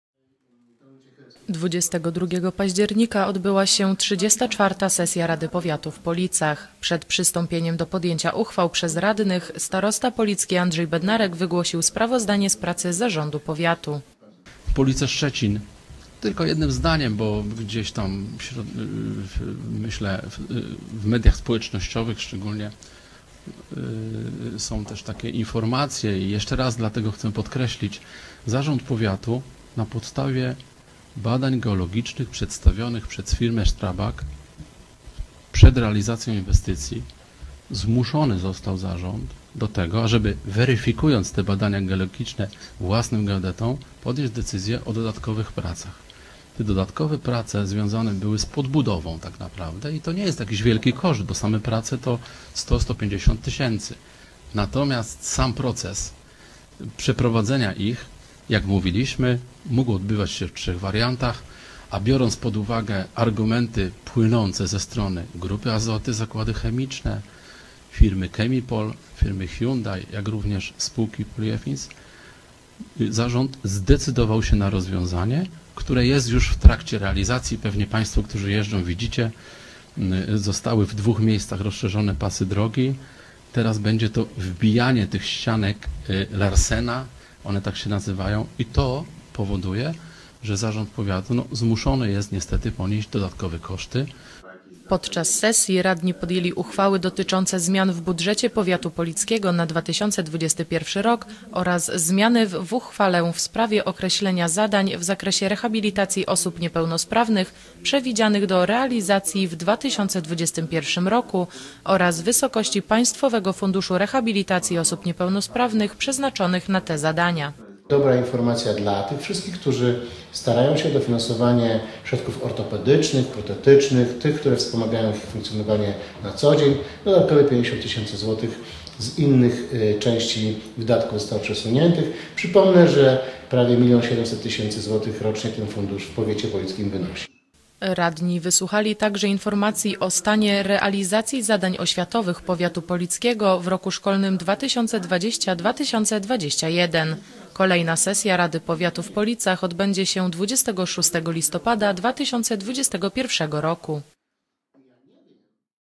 XXXIV sesja Rady Powiatu w Policach
Wiadomości z powiatu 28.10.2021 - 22 października odbyła się XXXIV sesja Rady Powiatu w Policach. Przed przystąpieniem do podjęcia uchwał przez radnych Starosta Policki Andrzej Bednarek wygłosił sprawozdanie z pracy Zarządu Powiatu.